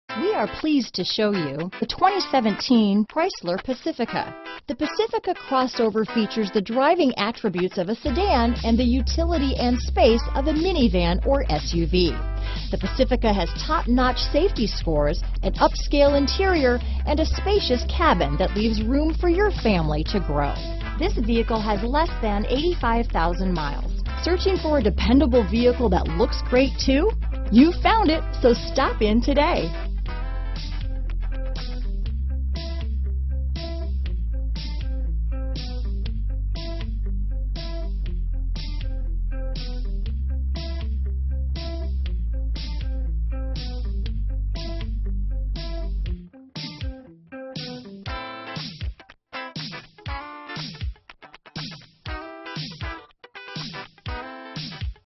2017 Chrysler Pacifica Ocala, The Villages, Brooksville, Chiefland, Homosassa FL 22024A
Granite Crystal Metallic Clearcoat  2017 Chrysler Pacifica available in Homosassa, Florida at Eagle Buick GMC. Servicing the Ocala, The Villages, Brooksville, Chiefland, FL area.
2017 Chrysler Pacifica Touring-L Plus - Stock#: 22024A - VIN#: 2C4RC1EG3HR622619